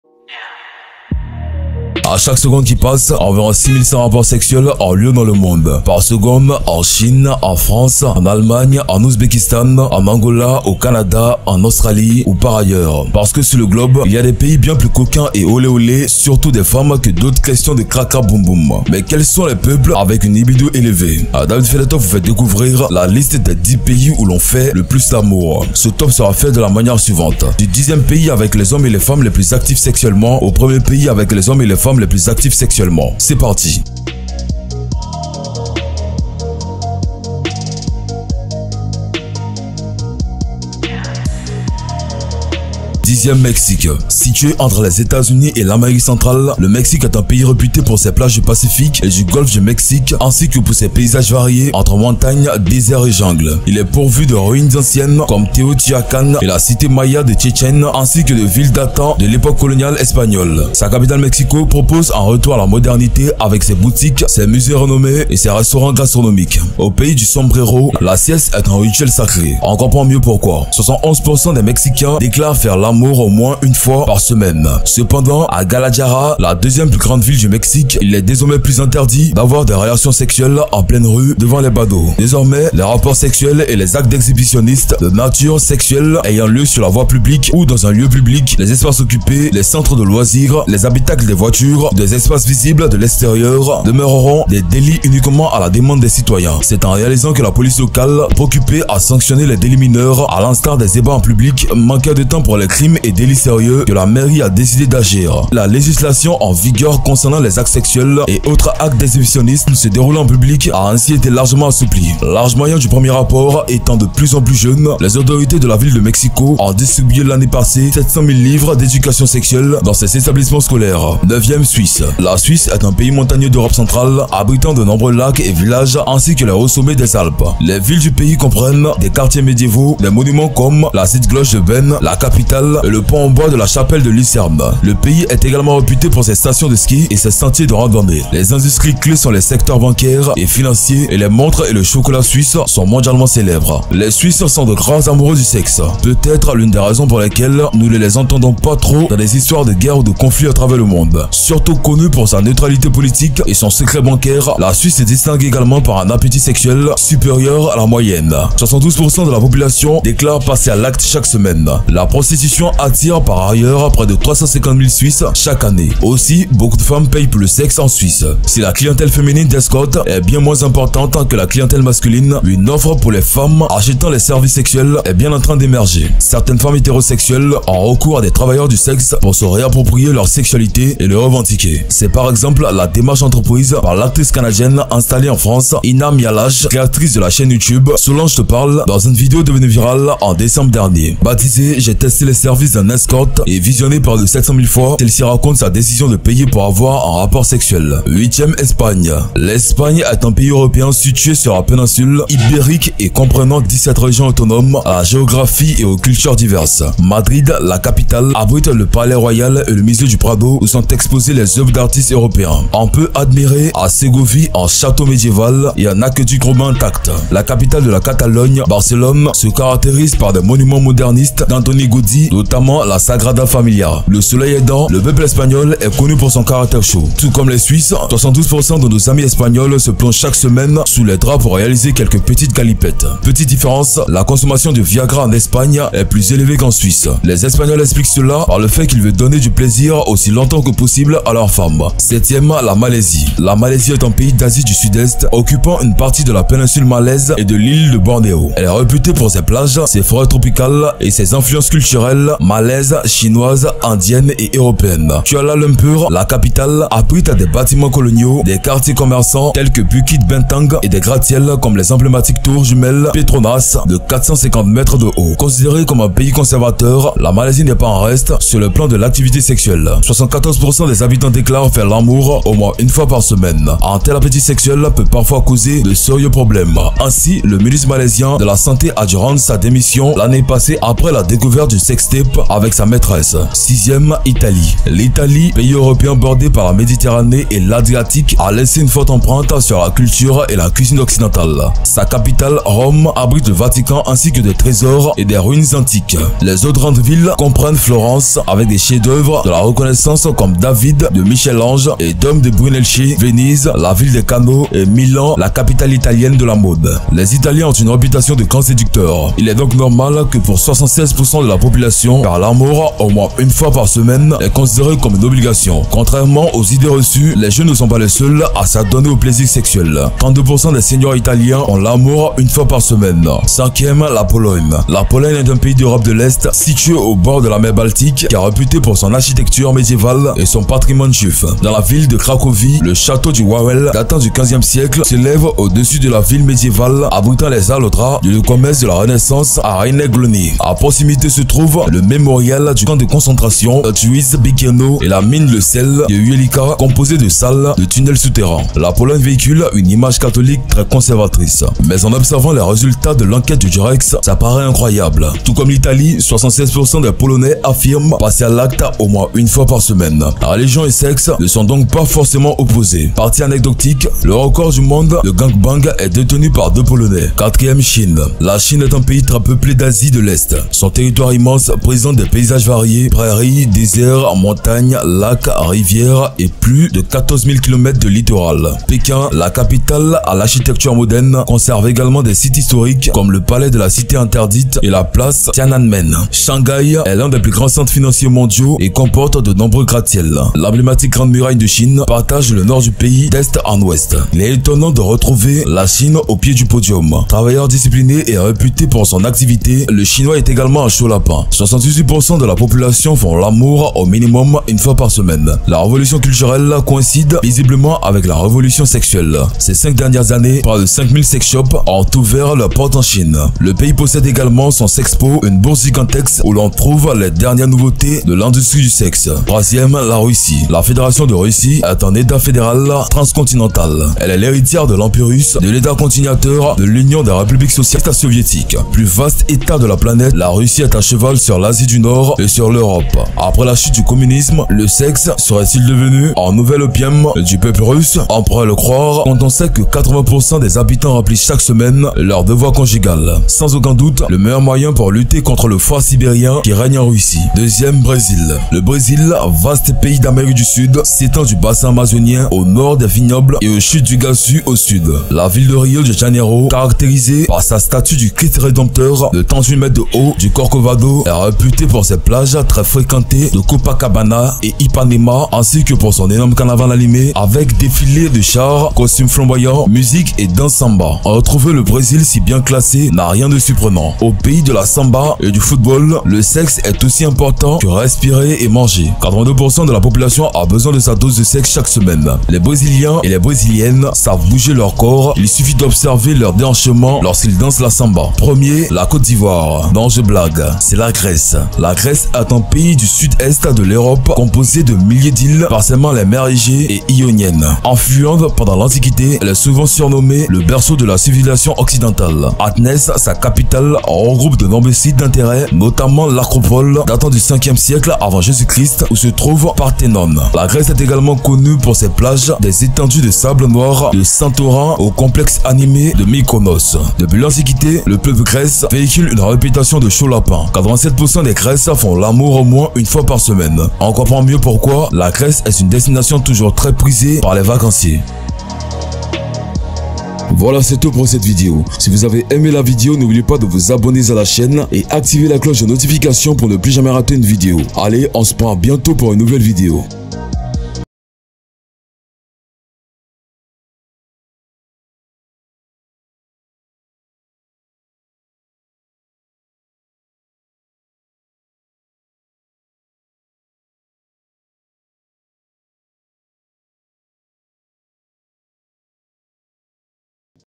Les femmes de ce pays aiment beaucoup le S3XE - DavidFaitDesTops
A chaque seconde qui passe, environ 6100 rapports sexuels ont lieu dans le monde. Par seconde... En Chine, en France, en Allemagne, en Ouzbékistan, en Angola, au Canada, en Australie... ou pas d'ailleurs ! Parce que sur le globe, il y a des pays bien plus coquins et olé olé surtout des femmes que d'autres question crac-crac... Boum, boum. Mais quels sont donc les peuples avec une libido élevée? DavidFaitDesTops vous fait découvrir, la liste des 10 pays où l'on fait le plus l'amour. Ce top sera fait de la manière suivante, Du 10 ème pays avec les hommes et les femmes les plus actifs sexuellement, au premier pays avec les hommes et les femmes les plus actifs sexuellement. C'est parti.